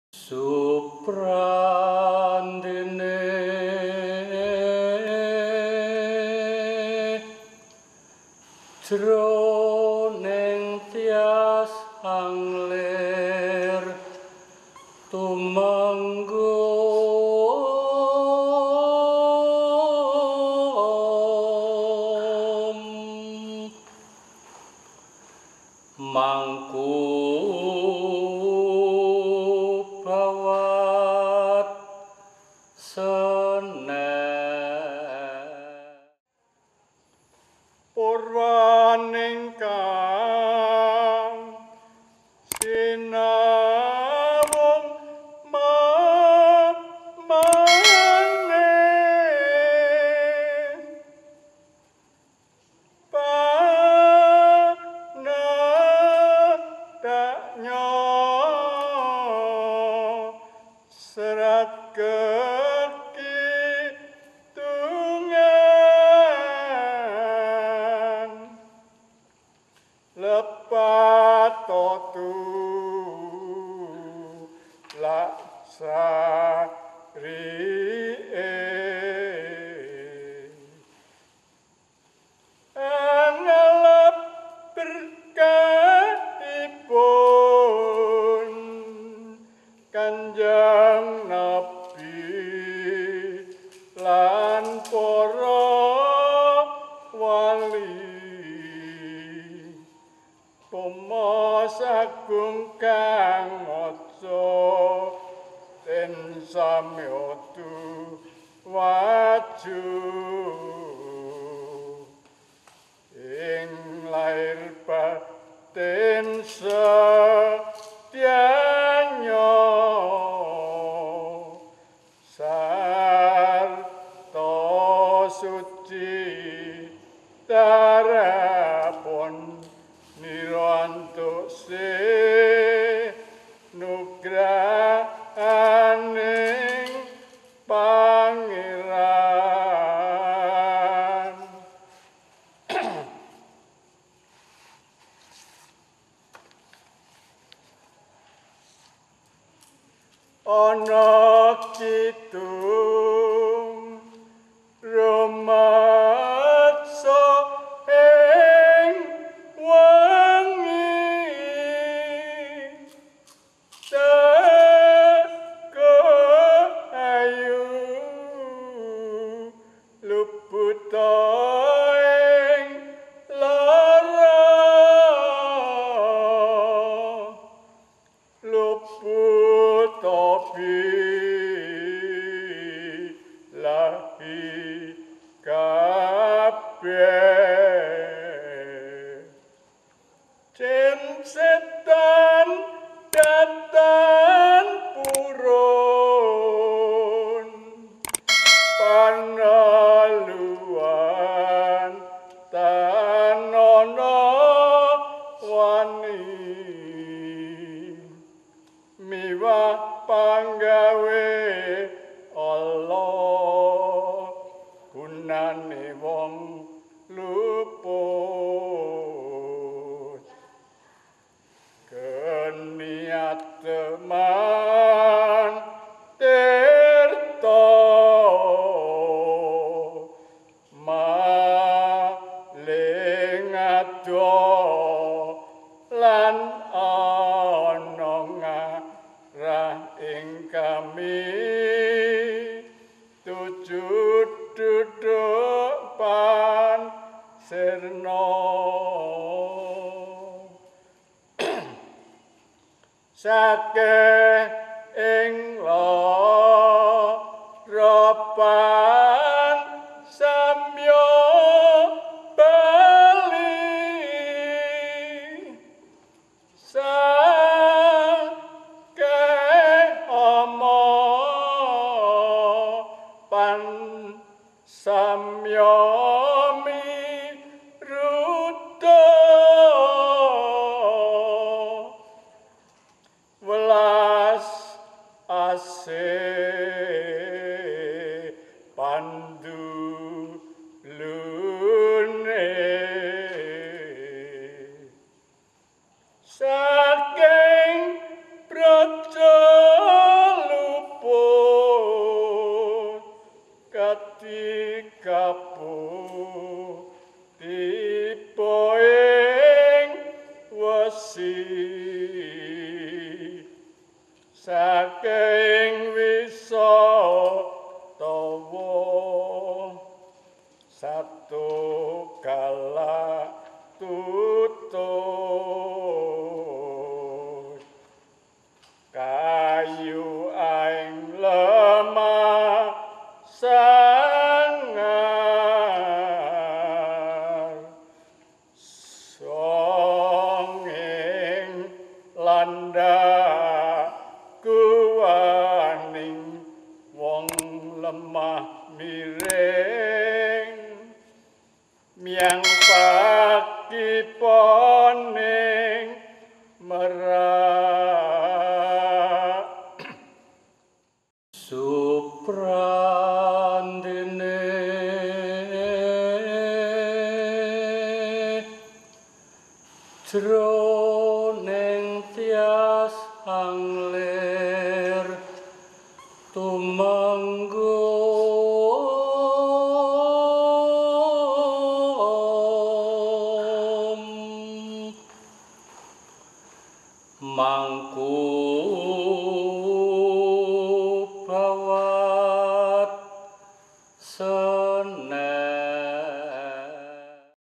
MOCOPATAN / MACAPATAN DALEM KARATON || #1 || KARATON SURAKARTA HADININGRAT
Mocopatan adalah seni melagukan atau menembangkan syair mocopat yang
dilakukan oleh sejumlah orang secara bergiliran. Mocopat merupakan tembang yang dominan dipergunakan dalam segala jenis kesenian Jawa, seperti karawitan, wayang kulit, wayang orang, kethoprak, ludruk, jathilan, langen mandrawanara dan lain-lain. Sejak jaman Pajang hingga Kasunanan Surakarta dan Kasultanan Yogyakarta, sastra mocopat merupakan sastra Jawa yang paling dominan. Hal itu dibuktikan dengan selalu munculnya mocopat dalam setiap seni pertunjukan yang ada.
Pada umumnya mocopat diartikan sebagai moco papat-papat (membaca empat-empat), yaitu cara membaca yang terhubung dengan tiap empat suku kata.
Namun ini bukan satu-satunya arti dari mocopatan. Definisi lain dari seorang pakar Sastra Jawa, Arps menguraikan beberapa arti lain di dalam bukunya Tembang in two tradition. Selain yang telah disebut di atas ini, arti lainnya ialah bahwa -pat merujuk kepada jumlah tanda diakritis (sandhangan) dalam aksara Jawa yang relevan dalam penembangan mocopat